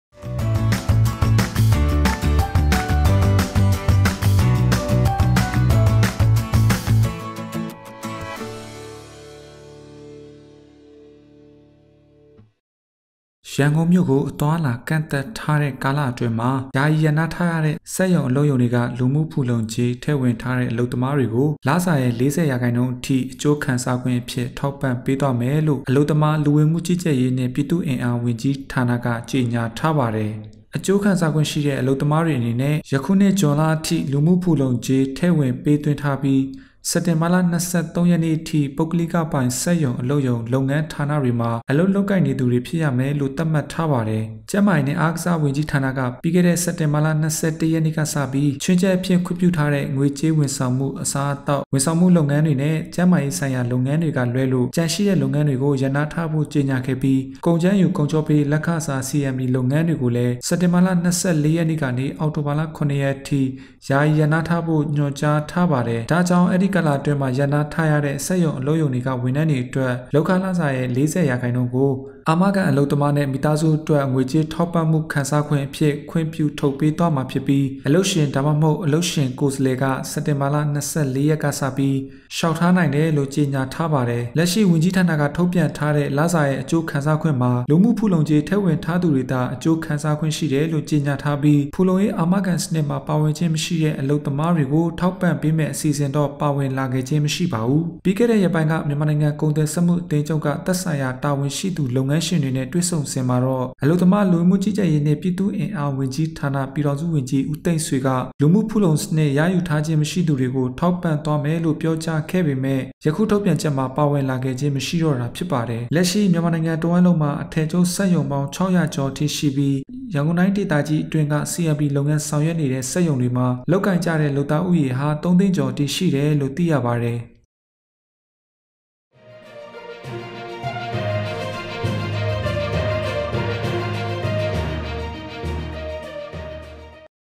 BETV Business l လူမှုဖူလုံကြေး ထည့်ဝင်ထားသည့် လုပ်သားများကို လစာ၏ ၄၀% အစိုးရ ထောက်ပံ့မည်
ယခုနှစ် ဇွန်လအထိ လူမှုဖူလုံကြေး ထည့်ဝင်ထားသည့် လုပ်သားများကို လစာ၏ ၄၀% အစိုးရ ထောက်ပံ့မည်

ရန်ကုန်မြို့ကို အသွားအလာ ကန့်သတ်ထားတဲ့ ကာလအတွင်းမှာ ယာယီရပ်နားထားရတဲ့ စက်ရုံ၊ အလုပ်ရုံတွေက လူမှုဖူလုံရေး ထည့်ဝင်ထားတဲ့ အလုပ်သမားတွေကို လစာရဲ့ (၄၀) ရာခိုင်နှုန်းအထိ အကျိုးခံစားခွင့်အဖြစ် ထောက်ပံ့ပေးသွားမယ်လို့ အလုပ်သမား၊ လူဝင်မှုကြီးကြပ်ရေးနှင့် ပြည်သူ့အင်အားဝန်ကြီးဌာနက သတင်းထုတ်ပြန်ထားပါတယ်။ အကျိုးခံစားခွင့် ရရှိတဲ့ အလုပ်သမားတွေအနေနဲ့ ပြီးခဲ့တဲ့ ယခုနှစ်၊ ဇွန်လအထိ လူမှုဖူလုံကြေး ထည့်ဝင်ပေးသွင်းထားပြီး စက်တင်ဘာလ ၂၃ ရက်နေ့အထိ ပုဂ္ဂလိကပိုင် စက်ရုံ၊ အလုပ်ရုံ၊ လုပ်ငန်းဌာနတွေမှာ အလုပ်လုပ်ကိုင်နေသူတွေ ဖြစ်ရမယ်လို့ သတ်မှတ်ထားပါတယ်။ ကျန်းမာရေးနှင့် အားကစားဝန်ကြီးဌာနက ပြီးခဲ့တဲ့ စက်တင်ဘာလ ၂၁ ရက်နေ့ကစပြီး ငွေကြေးဝန်ဆောင်မှု၊ အစားအသောက်၊ ဝန်ဆောင်မှုလုပ်ငန်းနဲ့ ကျန်းမာရေးဆိုင်ရာ လုပ်ငန်းတွေက လွဲလို့ ကျန်ရှိတဲ့ လုပ်ငန်းတွေကို ရပ်နားထားဖို့ ကြေညာခဲ့ပြီး ကုန်ကြမ်းယူ ကုန်ချောပေး လက်ခစား - CMP လုပ်ငန်းတွေကိုလည်း စက်တင်ဘာ (၂၄) ရက်ကနေ အောက်တိုဘာ (၇) ရက်နေ့အထိ ယာယီရပ်နားထားဖို့ ညွှန်ကြားထားပါတယ်။ ဒါ့ကြောင့် အဲ့ဒီကာလအတွင်းမှာ ရပ်နားထားရတဲ့ စက်ရုံ၊အလုပ်ရုံတွေက ဝန်ထမ်းတွေအတွက် လုပ်ခလစာရဲ့ (၄၀) ရာခိုင်နှုန်းကို အာမခံအလုပ်သမားနဲ့ မိသားစုအတွက် ငွေကြေးထောက်ပံ့မှု ခံစားခွင့်အဖြစ် ခွင့်ပြုထုတ်ပေးသွားမှာဖြစ်ပြီး အလုပ်ရှင် ဒါမှမဟုတ် အလုပ်ရှင်ကိုယ်စားလှယ်က စက်တင်ဘာ ၂၄ရက်က စတင်ပြီး လျှောက်ထားနိုင်တယ်လို့ ကြေညာထားပါတယ်။ လက်ရှိ ဝန်ကြီးဌာနက ထုတ်ပြန်ထားတဲ့ လစာရဲ့ အကျိုးခံစားခွင့်မှာ လူမှုဖူလုံကြေးထည့်ဝင်ထားသူတွေသာ ရယူခွင့်ရှိတယ်လို့ ကြေညာထားပြီး ဖူလုံရေးအာမခံစနစ်မှာ ပါဝင်ခြင်းမရှိတဲ့ အလုပ်သမားတွေကို ထောက်ပံ့ပေးမယ့် အစီအစဉ်တော့ ပါဝင်လာခဲ့ခြင်းမရှိပါဘူး။ ပြီးခဲ့တဲ့ ရက်ပိုင်းက မြန်မာနိုင်ငံကုန်သည်၊စက်မှုအသင်းချုပ်က သက်ဆိုင်ရာတာဝန်ရှိသူ လုပ်ငန်းရှင်တွေနဲ့ တွေ့ဆုံစဉ်မှာတော့ အလုပ်သမား၊ လူဝင်မှုကြီးကြပ်ရေးနှင့် ပြည်သူ့အင်အားဝန်ကြီးဌာန၊ ပြည်ထောင်စုဝန်ကြီး ဦးသိန်းဆွေက လူမှုဖူလုံစနစ် ရယူထားခြင်းမရှိသူတွေကို ထောက်ပံ့သွားမယ်လို့ ပြောကြားခဲ့ပေမယ့် ယခုထုတ်ပြန်ချက်မှာ ပါဝင်လာခဲ့ခြင်းမရှိတာ ဖြစ်ပါတယ်။ လက်ရှိ မြန်မာနိုင်ငံတဝှမ်းလုံးမှာ အထည်ချုပ် စက်ရုံပေါင်း ၆၀၀ ကျော်ရှိပြီး  ရန်ကုန်တိုင်းဒေသကြီးအတွင်းက CMP လုပ်ငန်း ဆောင်ရွက်နေတဲ့ စက်ရုံတွေမှာ လုပ်ကိုင်ကြတဲ့ လုပ်သားဦးရေဟာ (၃) သိန်းကျော်အထိ ရှိတယ်လို့ သိရပါတယ်။